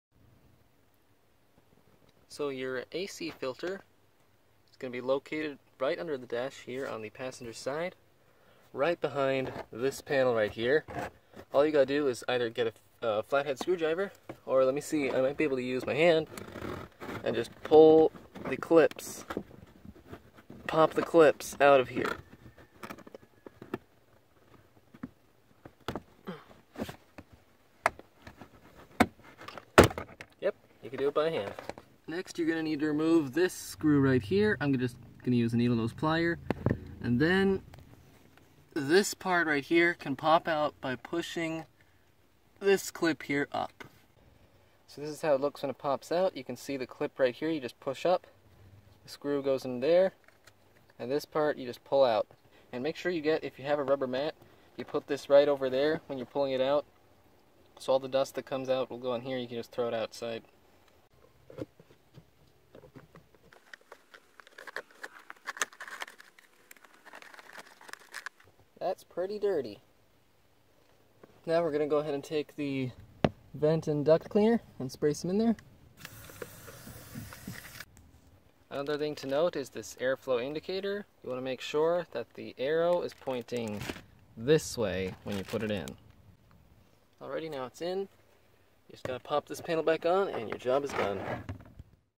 Cabin Air Filter Replacement on Dodge Dart
Cabin Air Filter
Air Freshener/Cleaner

Changing the cabin air filter on your Dodge Dart is very simple.